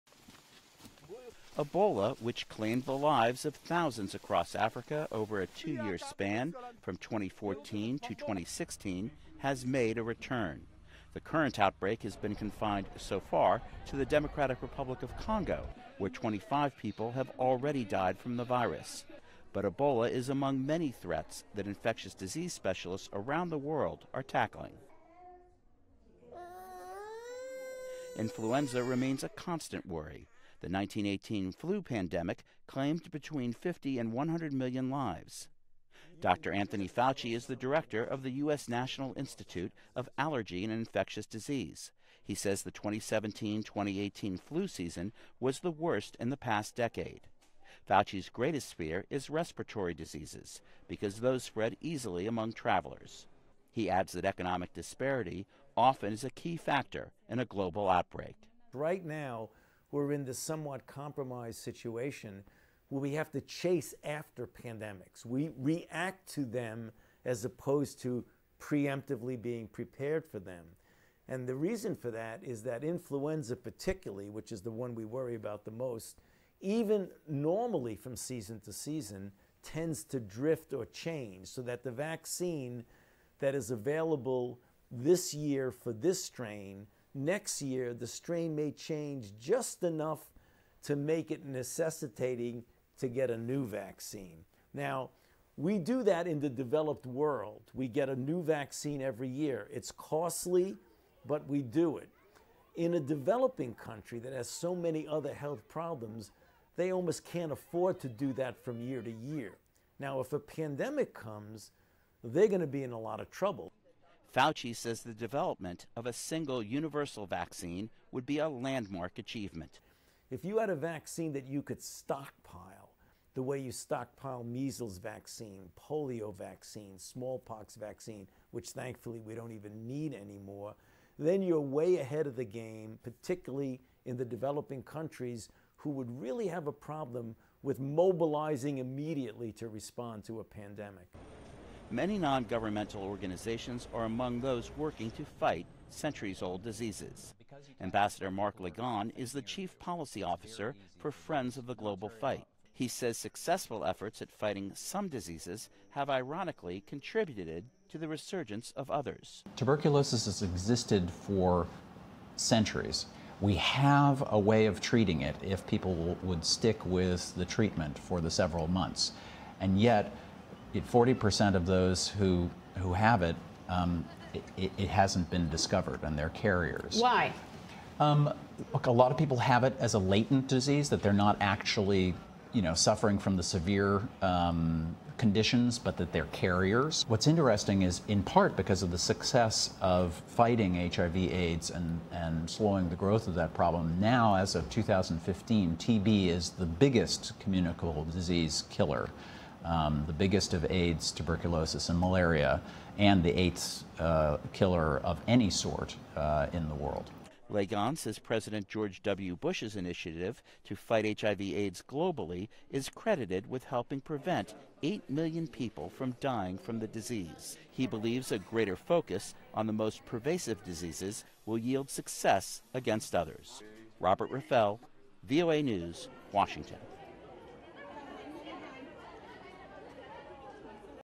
Despite Advances, Concerns About Pandemics Still Abound
Over the past decade, medical researchers and global initiatives have made significant advances to curb the spread of infectious diseases. Despite progress, experts say easier and faster human travel around the globe, combined with ever-mutating viruses, make the possibility of a pandemic a constant concern.